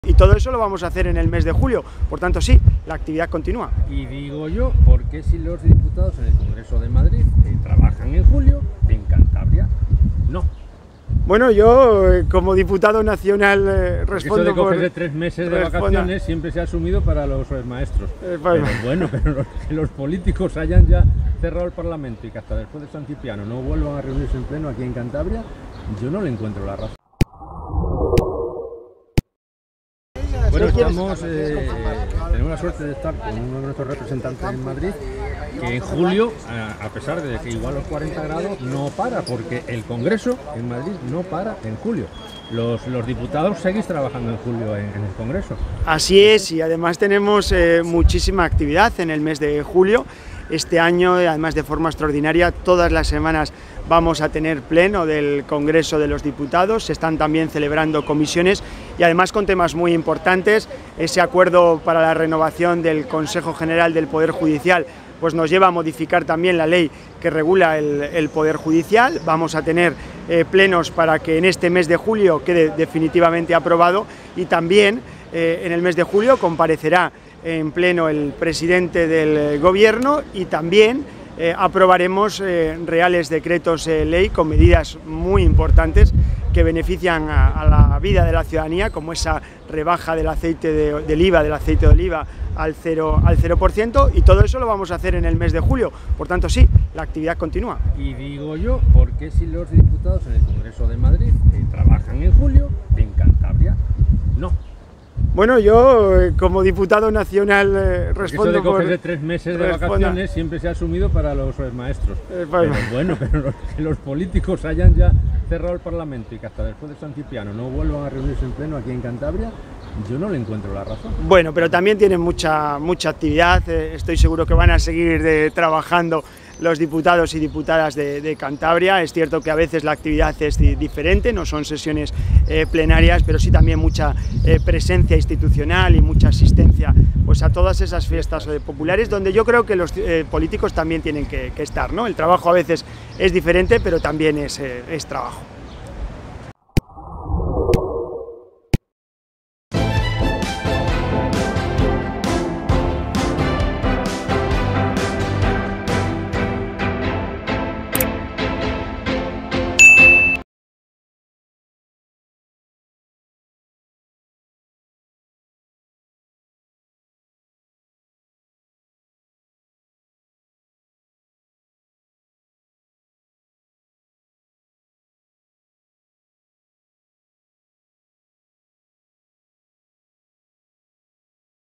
Mientras los diputados en Madrid trabajan los de Cantabria se van de fiesta
Pedro Casares defiende la labor de los diputados del Parlamento de Cantabria con evidente sentido corporativista.
Mientras los diputados en el Congreso de los Diputados de Madrid trabajan intensamente en el mes de julio los Diputados en Cantabria no celebran plenos y, eso si, se esfuerzan mucho en ir de fiesta en fiesta